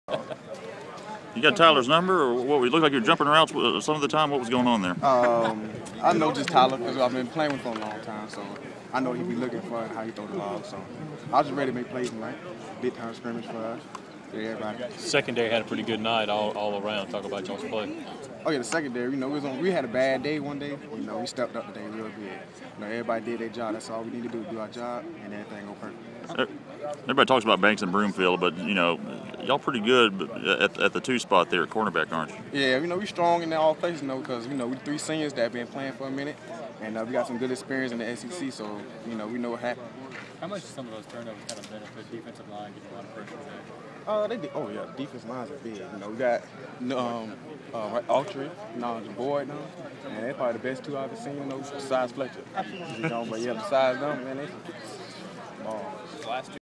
Darius Slay 4.13.12
Mississippi State cornerback Darius Slay talks after his team's scrimmage. Slay had three interceptions. Friday, April 13, 2012.